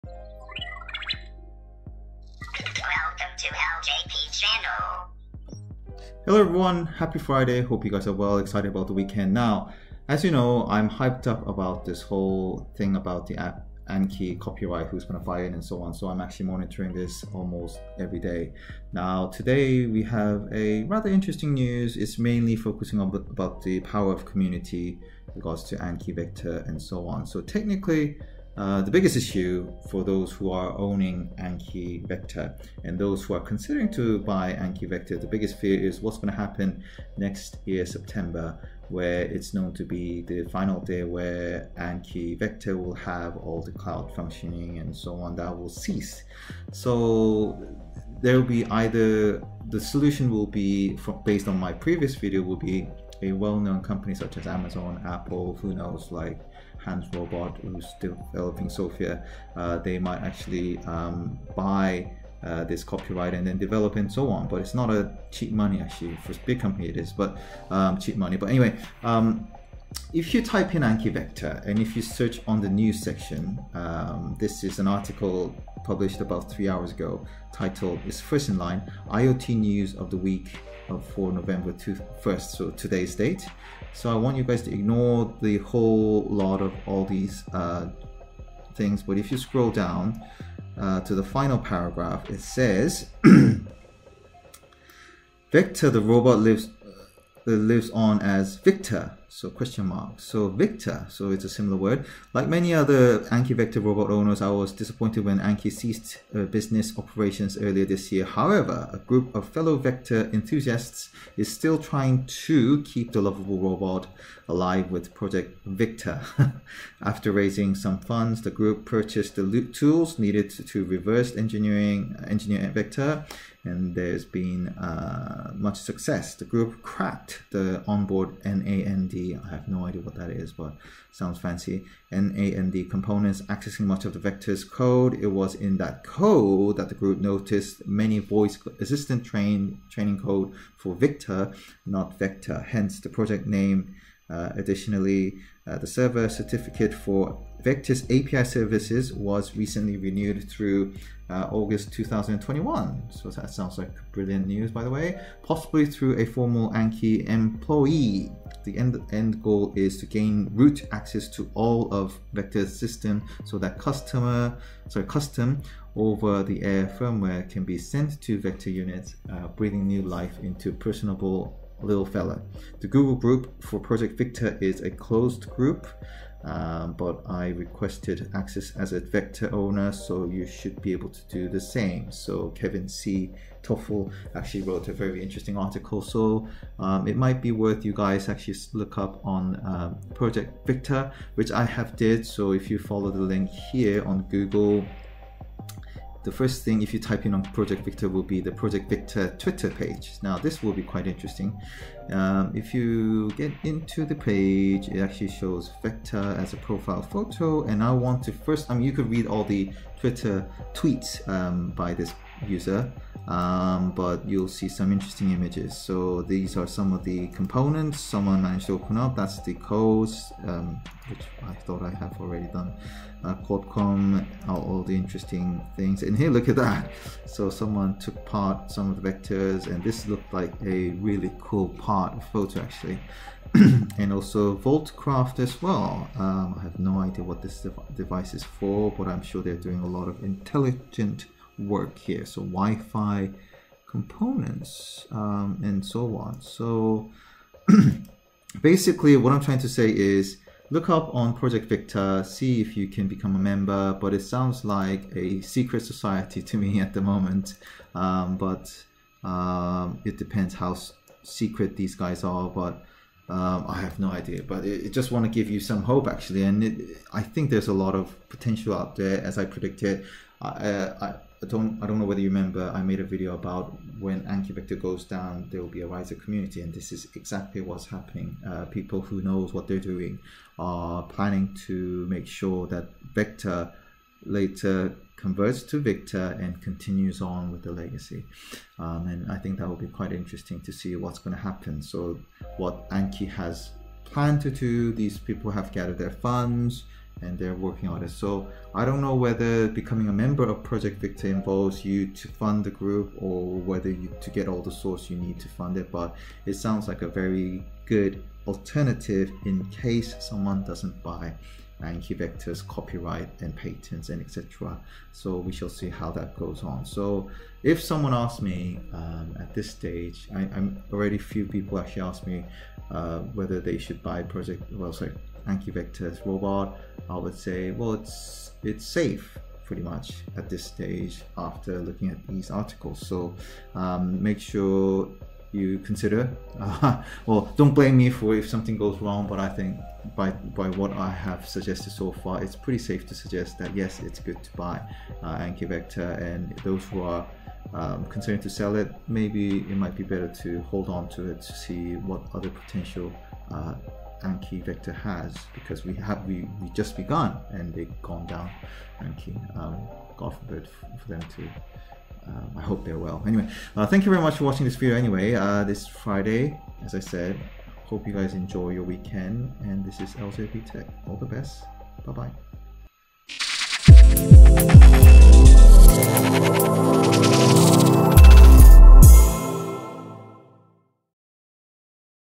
Project Victor, Not Vector (Anki, Rise Of Community - Part 6)
Related Article

IoT news of the week for Nov. 1, 2019

' a group of fellow Vector enthusiasts is trying to keep the lovable little robot alive with “Project Victor.” After raising some funds, the group purchased the tools needed to reverse engineer Vector. And there’s been much success. The group cracked the onboard NAND components, accessing much of Vector’s code. It was in that code that the group noticed many voice assistant training code for “Victor,” not Vector; hence the project name. Additionally, the server certificate for Vector’s API services was recently renewed through August 2021, possibly through a former Anki employee. The end goal is to gain root access to all of Vector’s systems so that custom, over-the-air firmware can be sent to Vector units, breathing new life into the personable li’l fellow. The Google Group for Project Victor is a closed group, but I requested access as a Vector owner, so you should be able to do the same.'

 – Kevin C. Tofel